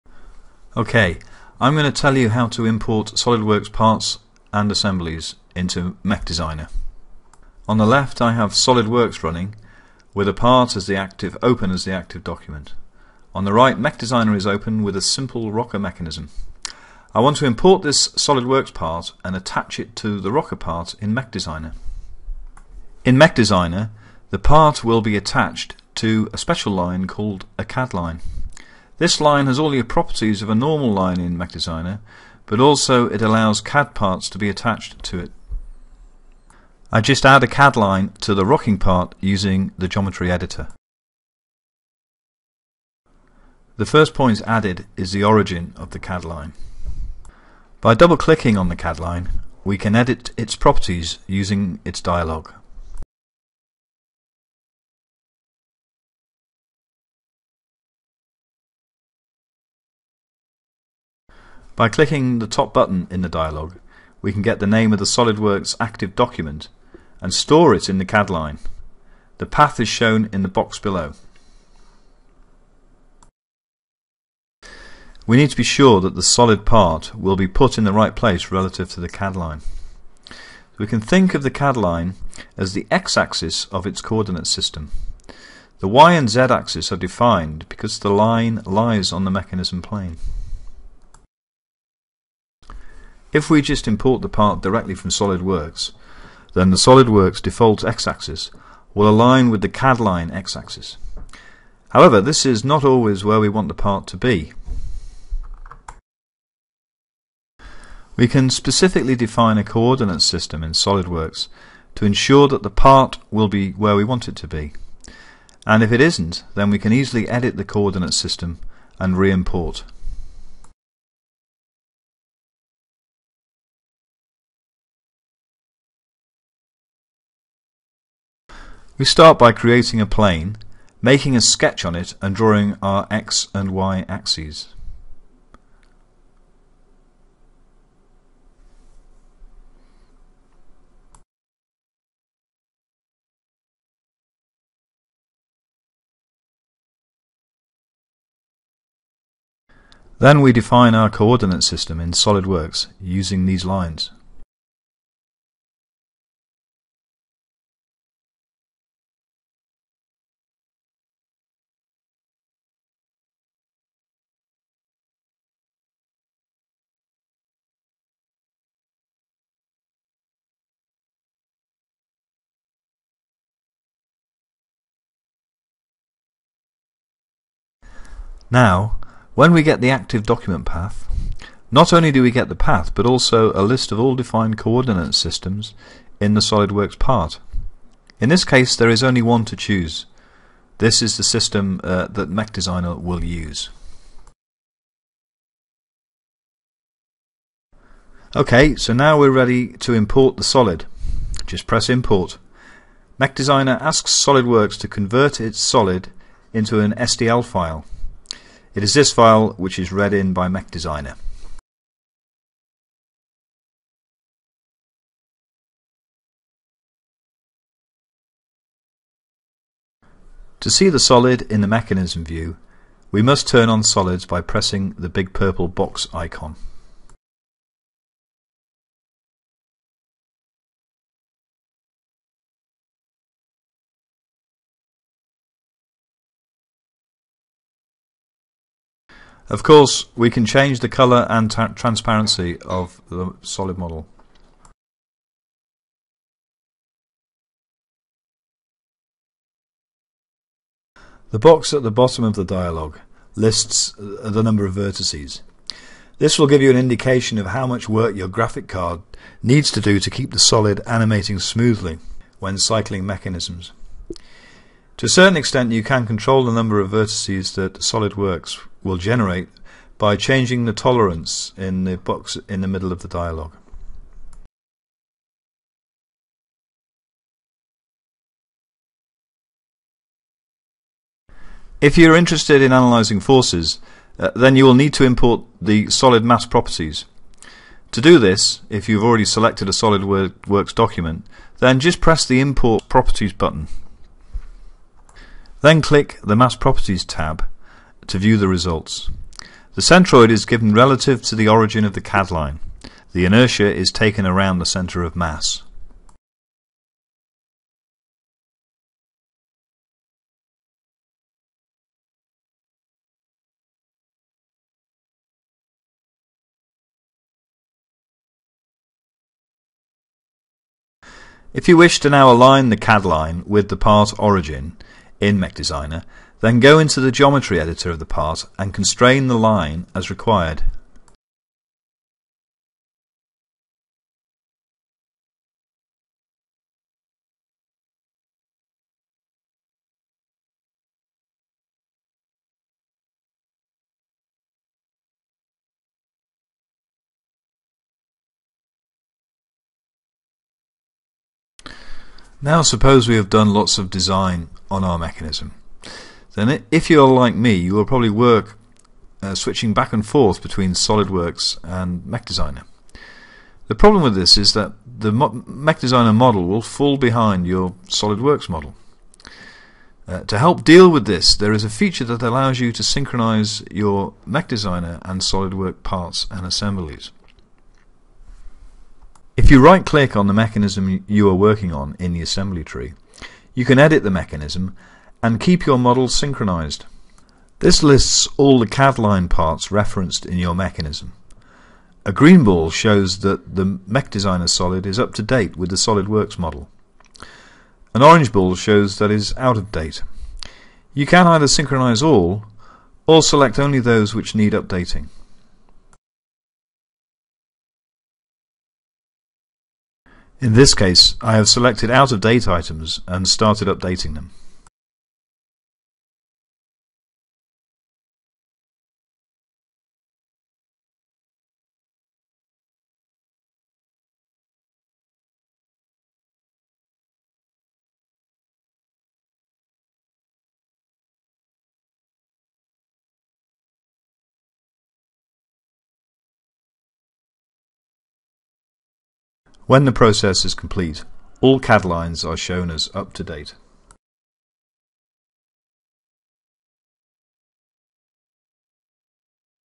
How to Import SolidWorks Parts into MechDesigner
Tutorial describing how to import parts and mass properties from SolidWorks into MechDesigner. You can use the power of MechDesigner to model the motion and configuration of many complex mechanisms in a machine, anduse the power of SolidWorks to model complex 3D shapes.